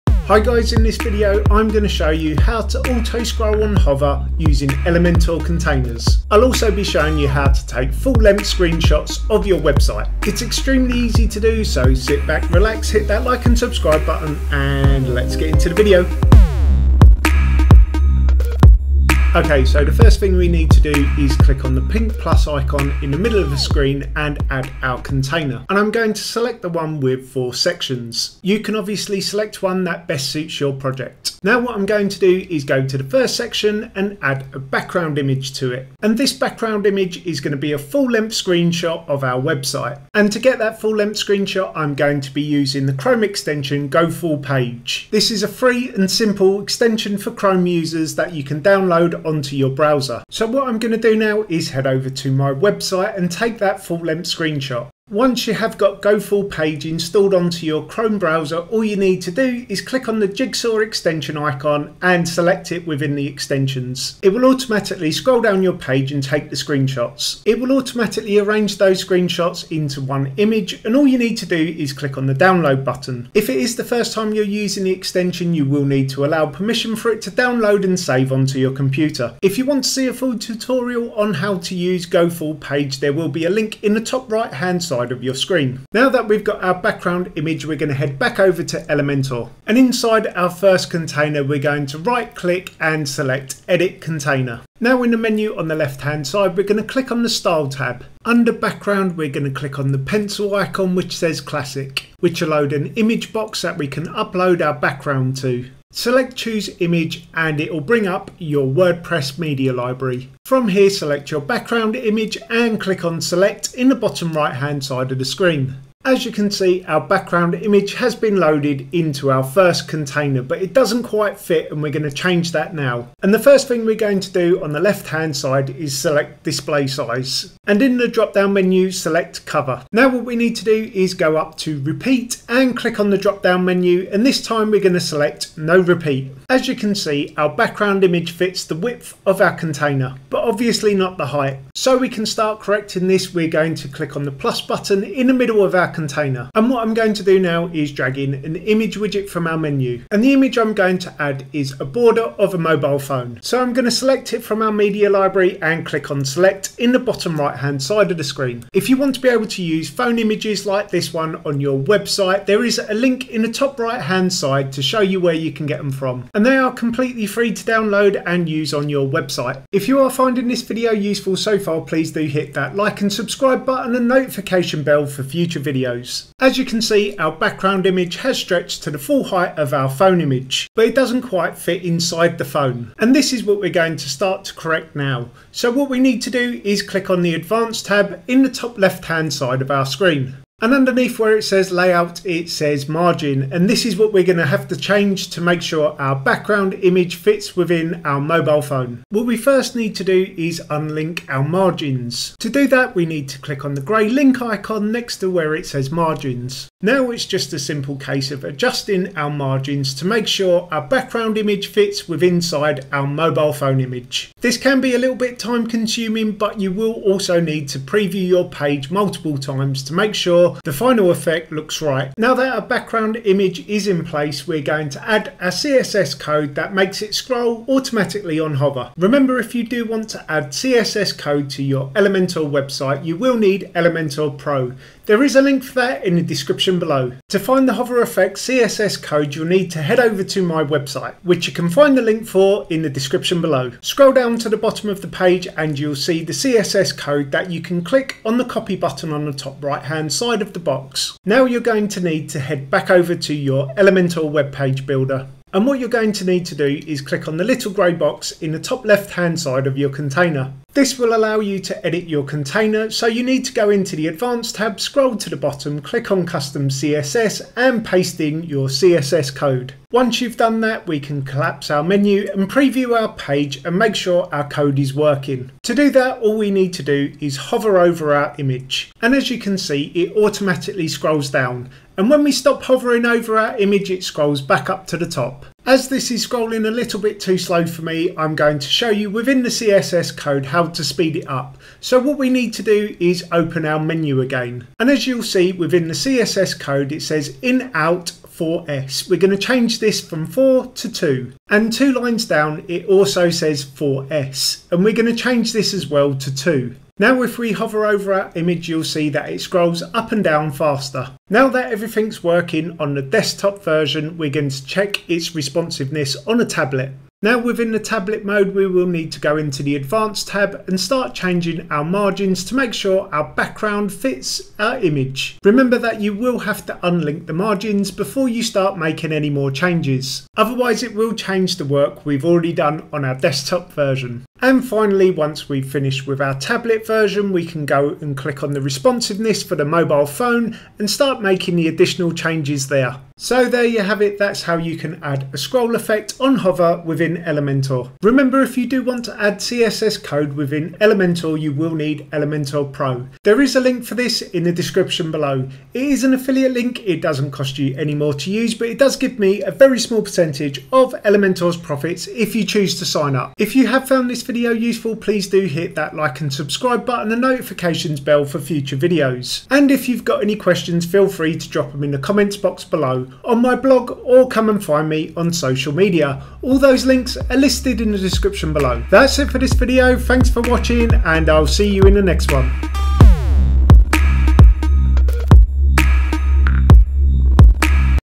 Portfolio Image Auto Scroll Effect Elementor FlexBox Containers
In this Video I will tell you how to add a Portfolio Image Auto Scroll Effect Elementor FlexBox Containers. I will also be showing you how to use GoFullPage and explain how to edit the CSS code.

For website hosting there is two options. SiteGround and NameHero.

00:00 - Intro - Portfolio Image Auto Scroll Effect Elementor FlexBox Containers
00:18 - How To Set Up Our Page For Auto Scroll
00:55 - How To Take A Full Length Screenshot Of Website Using GoFullPage
01:26 - How To Add A full Length Website Image To Website
02:20 - How To Add A Mobile Phone Boarder To Container
02:56 - How To Adjust Margins For Full Scroll Effect Image
03:43 - How To Add Portfolio Image Auto Scroll Effect CSS Code
04:42 - How To Edit The Auto Scroll CSS Code
05:14 - How To Edit Your Website Long Scroll Effect For Tablets
05:35 - How To Edit Your Website Long Scroll Effect For Mobile